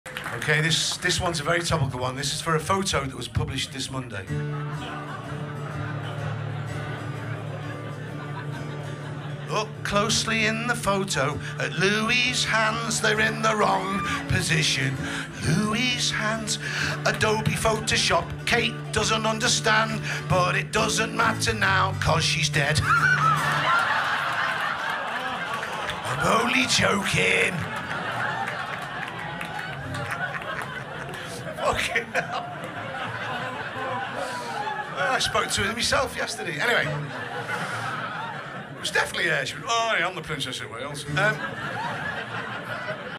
"Prince Louis' Hands (The Kate Middleton Photoshop Rabbit Hole"
This is my take on the Adobe Photoshop weirdness of the Kate Middleton Mothering Sunday doctored photograph. Reuters issued a Kill Notice on the photo, saying it had been Manipulated. Like The British Press and The Royal Family have never manipulated anything?